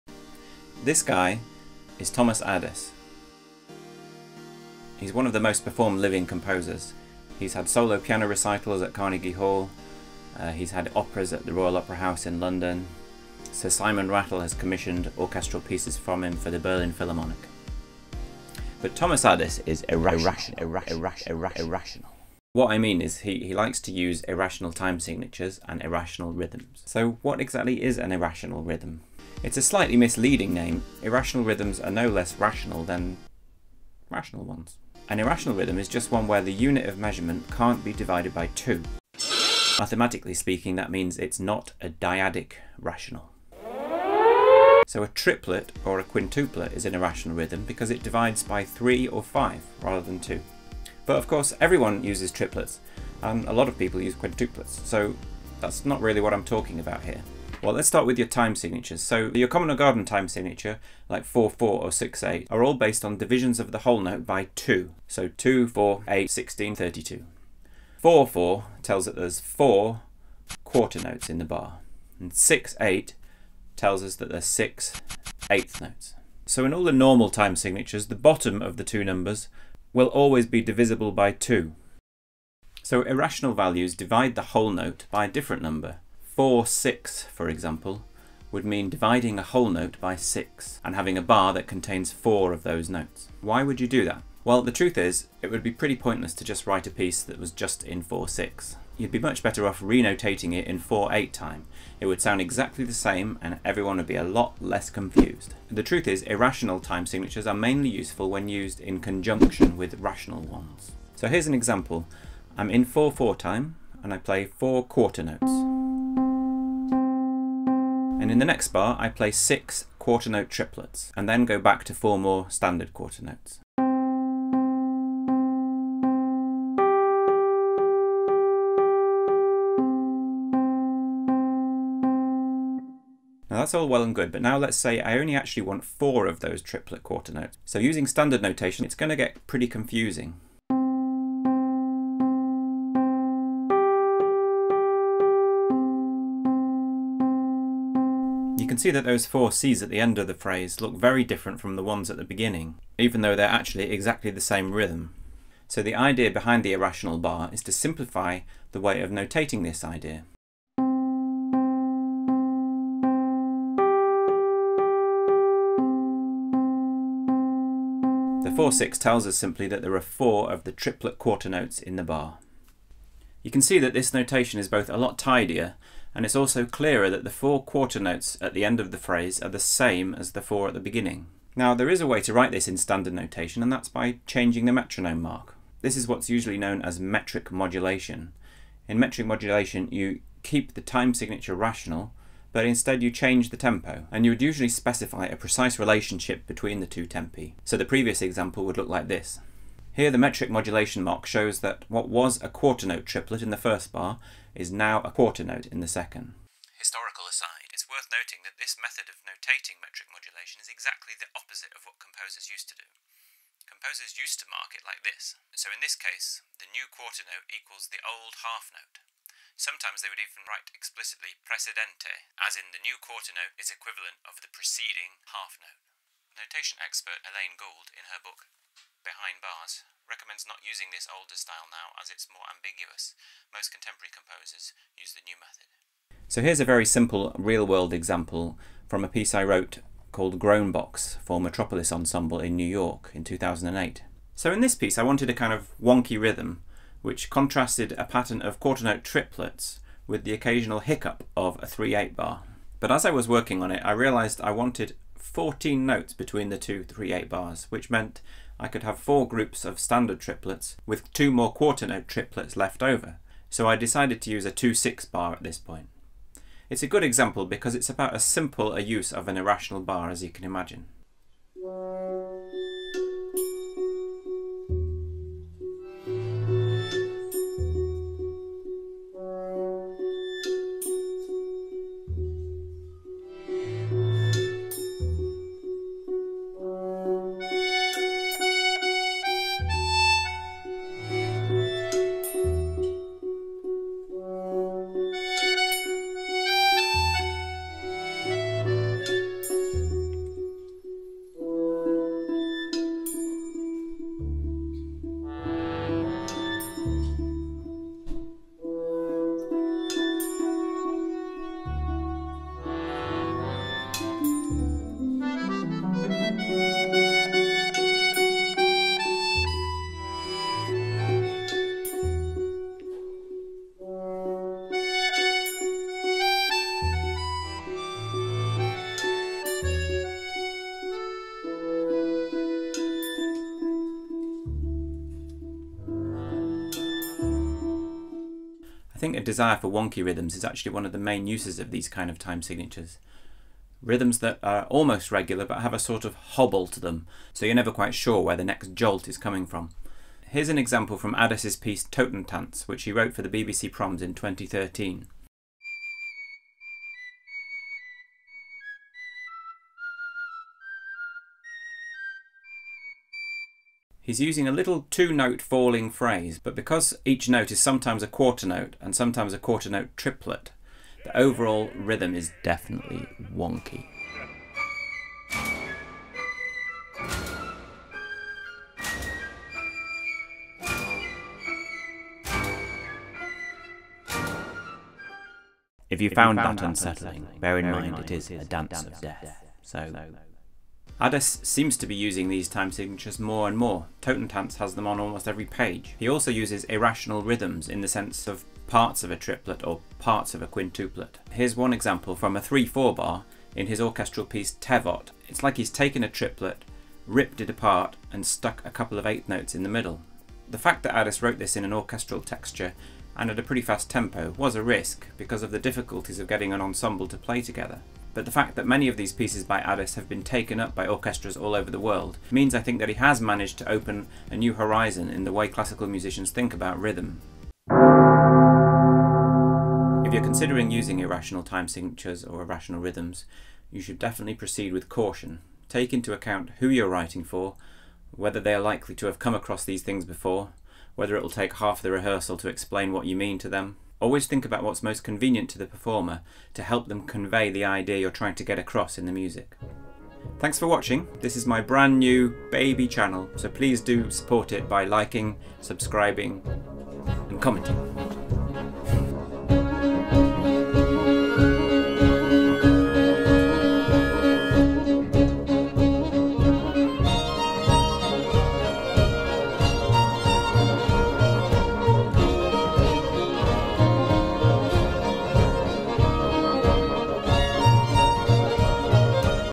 Irrational Time Signatures & Rhythms
Composer David Bruce discusses the use of irrational time signatures, and irrational rhythms. Why do composers use them and has their use by prominent composers like Thomas Ades made them more acceptible?

Research:
Elaine Gould's Behind Bars, a definitive guide to modern notation:
Huw Belling Masters Thesis "Thinking Irrational - Thomas Ades and new rhythms"